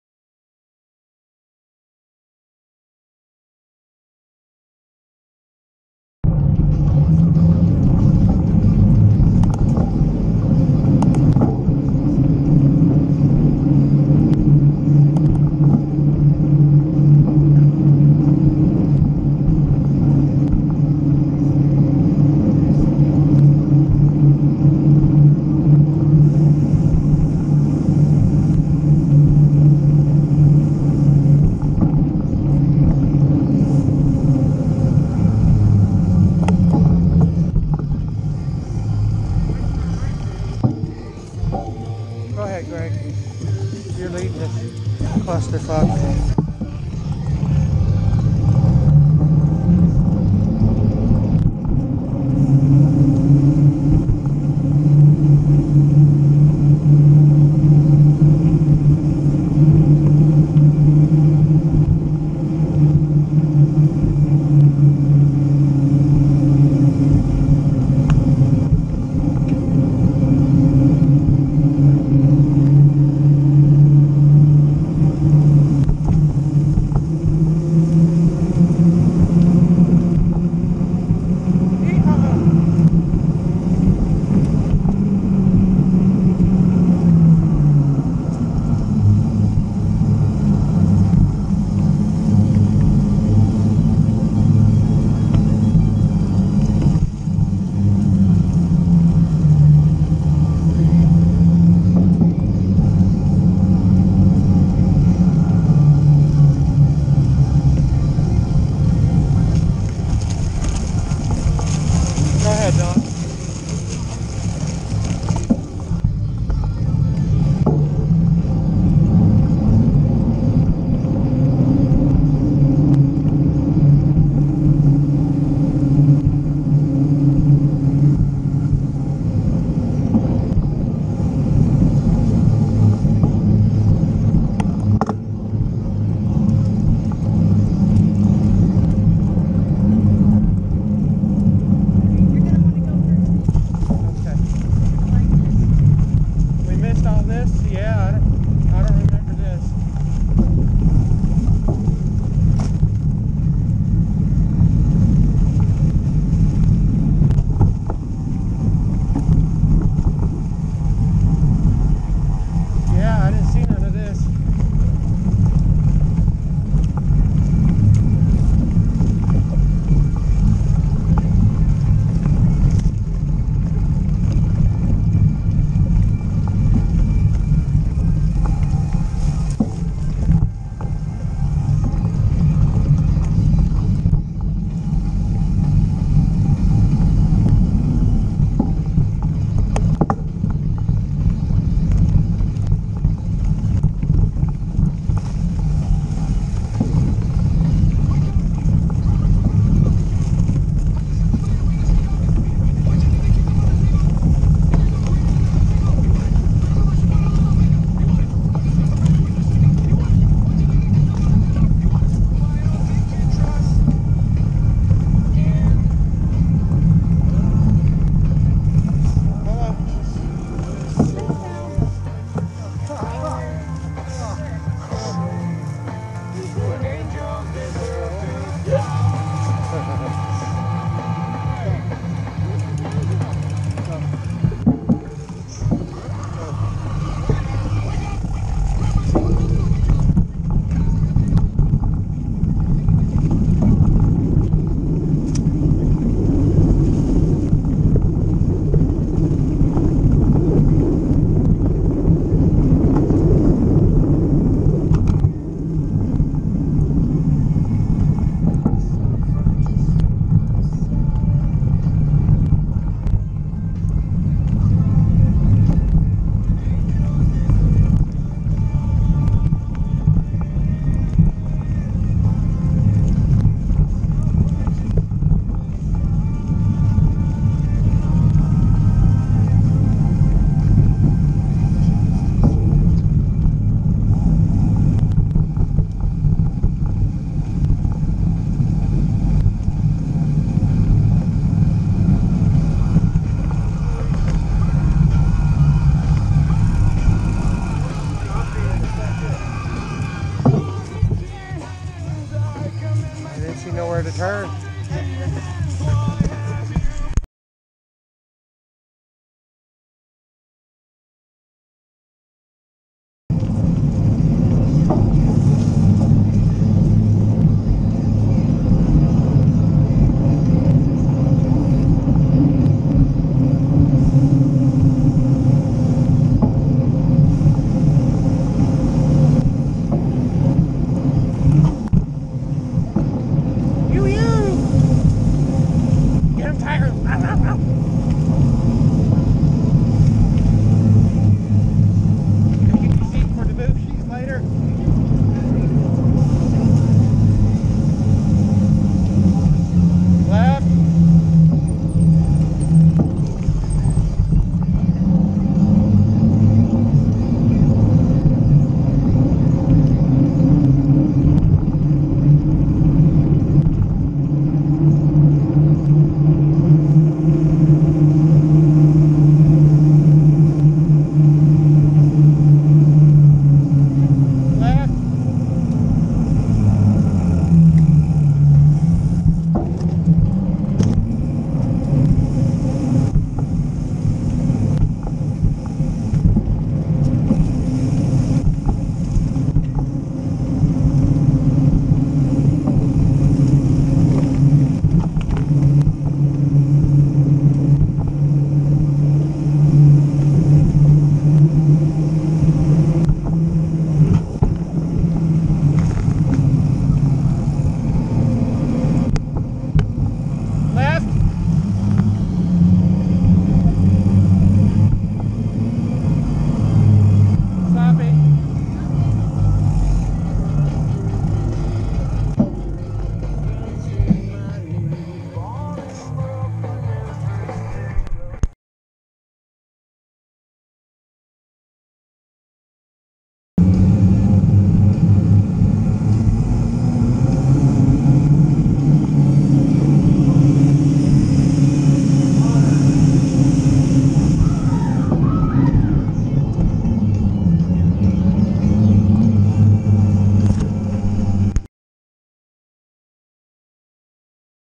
MR TOADS WILD RIDE
MIXED URBAN RIDE IN LONGWOOD,FLORIDA, MOUNTIAN BIKING